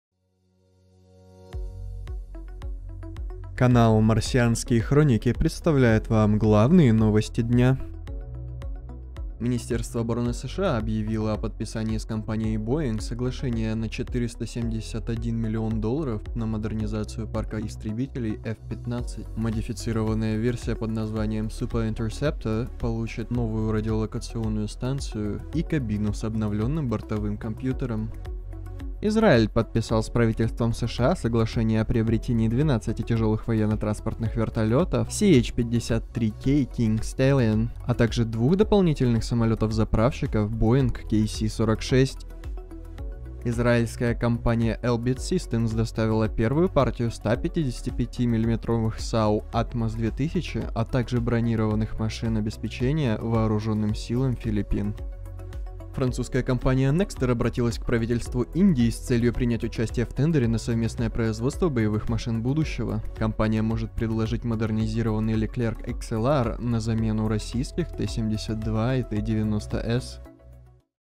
Франция вытесняет Т-90 из Индии | 03.01.2022
Подборка главных военных новостей от МХ